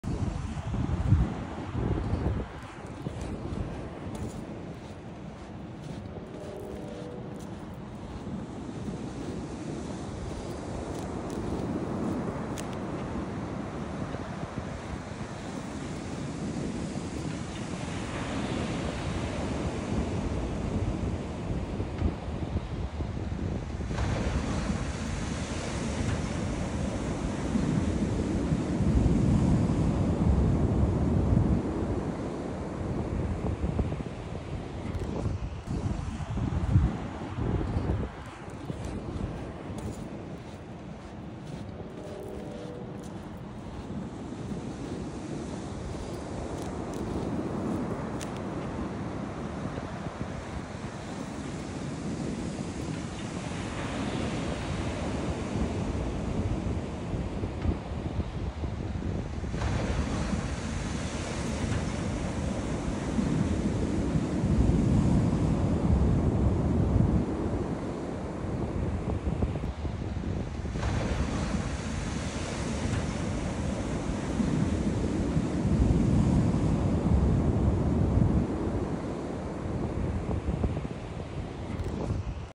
COMMERCIAL FISHING VESSEL RUNS AGROUND OUTSIDE OF MANASQUAN INLET
F/V Susan Rose ran aground on Point Pleasant Beach this morning around 5:15am — about 100 yards from Manasquan Inlet. All three crew members are okay.